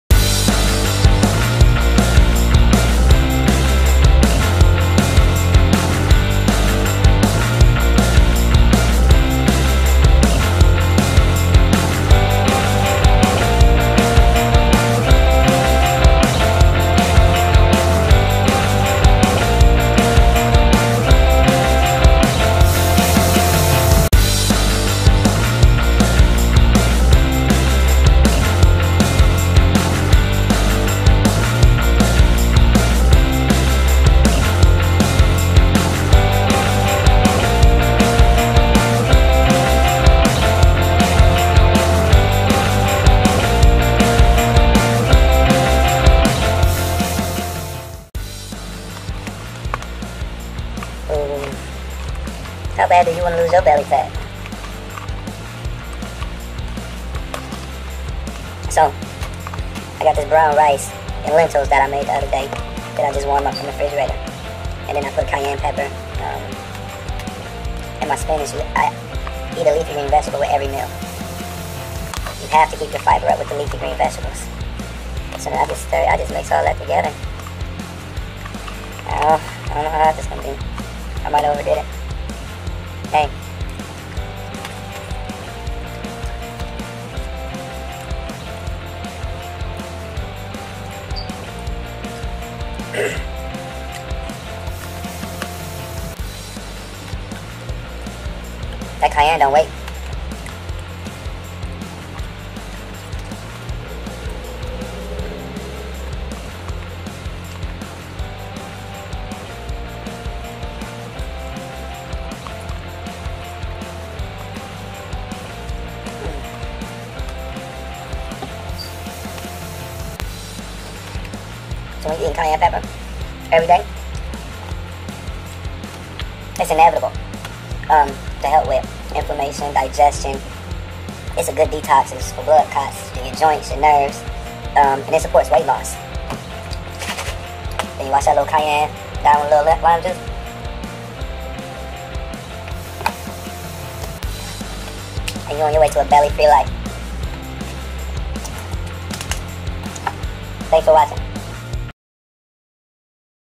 Belly Fat burning pre-workout meal
Brown rice lentils spinach cayenne pepper and balsamic vinegar dressing wash down with lime juice and spring water